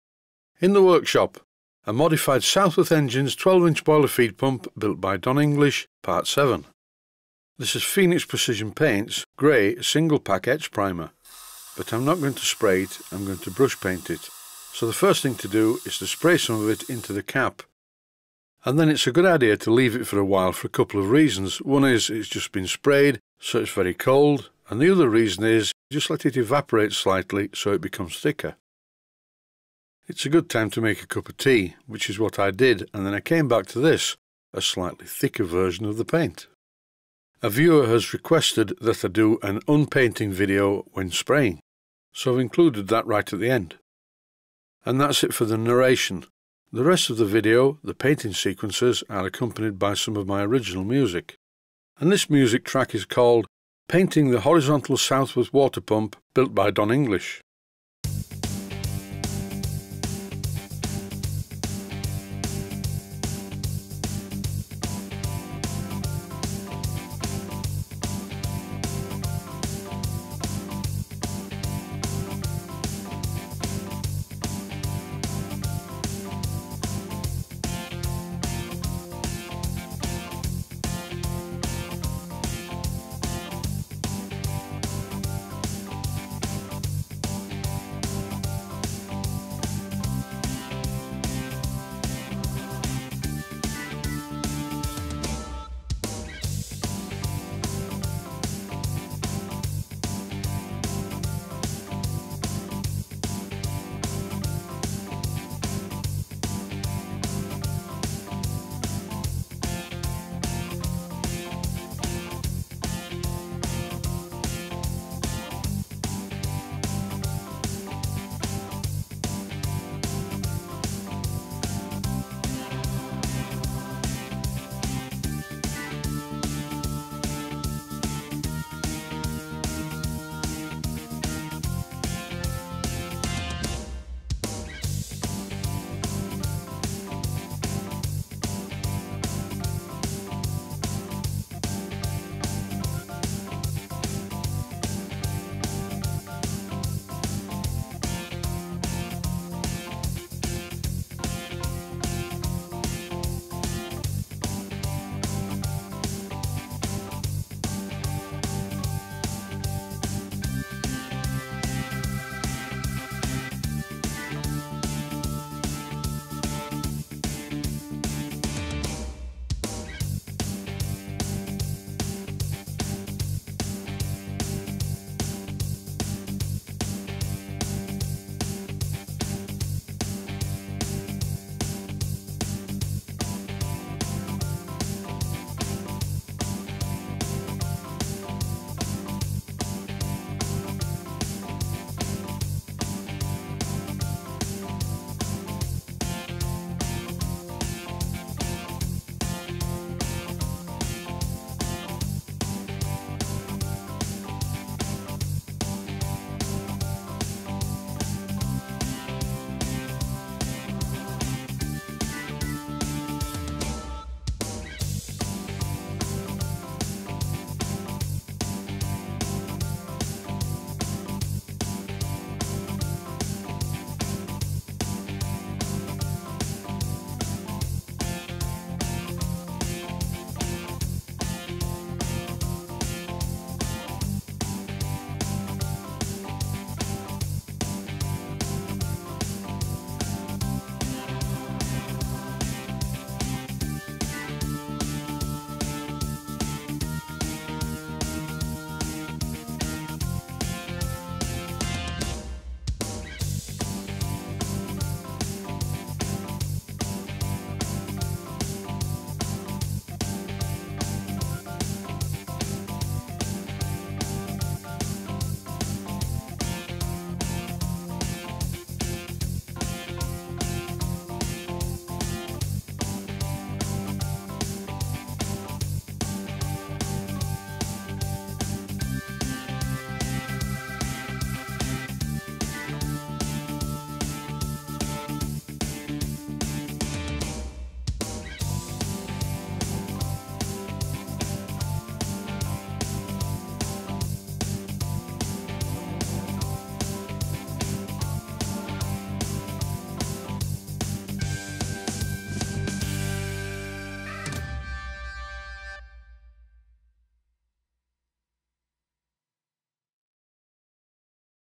A HORIZONTAL SOUTHWORTH 12 INCH BOILER FEED PUMP - PART #7
A Horizontal Southworth 12 Inch Boiler Feed Pump - Part #7 - Painting the Pump and parts with Etch Primer plus a bit of "unpainting" at the beginning and end of the video.